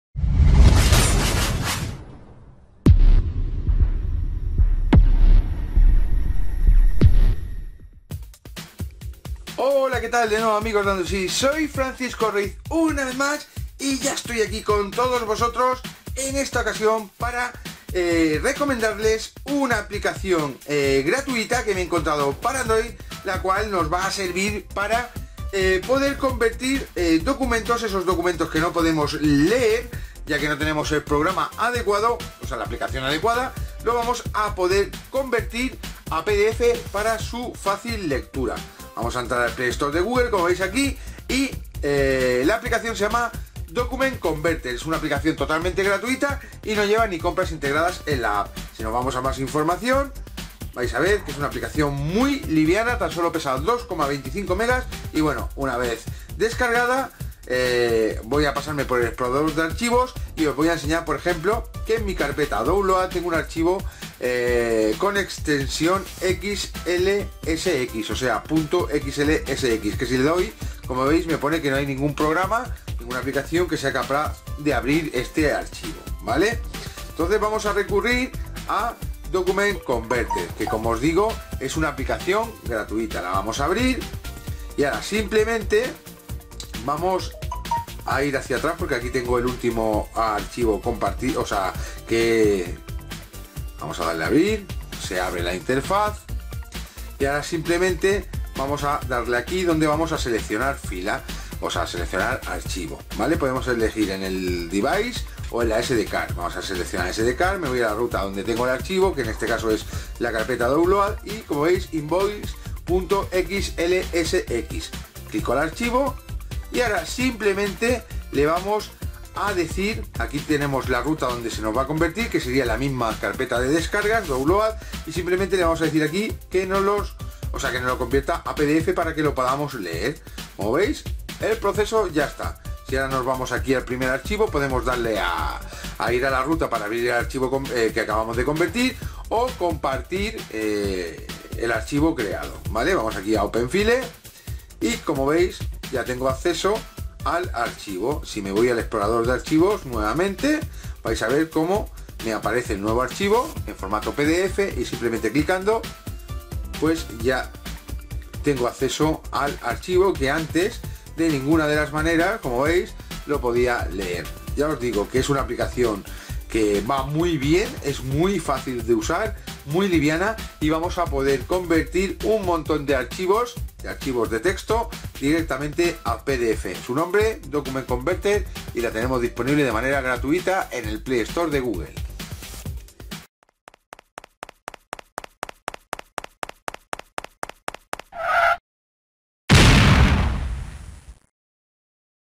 Cómo convertir todo tipo de documentos a PDF
Una de las cosas que más no pedís a través de mensajes en las diferentes redes sociales y el propio blog de Androidsis, es de alguna solución sencilla para poder convertir todo tipo de documentos a PDF, o lo que es lo mismo, que os recomendemos alguna aplicación para convertir cualquier tipo de Doc a PDF.

Dentro del Play Store de Google existen muchas aplicaciones que nos van a permitir conseguir convertir cualquier tipo de Doc a PDF, aunque la mayoría de ellas, o son demasiado complicadas de utilizar para la mayoría de los usuarios, o nos vienen muy limitadas a no ser que optemos por la versión de pago o PRO. Este no es el caso de la aplicación totalmente gratuita que hoy os pasamos a presentar, una aplicación que bajo el nombre de Document Converter y con tan solo hacer un par de clicks en la pantalla de nuestro Android, nos va a permitir convertir un buen número de formatos distintos de documentos al formato PDF. A continuación os explicamos todos los detalles para conseguir convertir Doc to PDF así como adjuntamos el link de descarga directo al Play Store de Google y el sencillo método de utilización de la aplicación.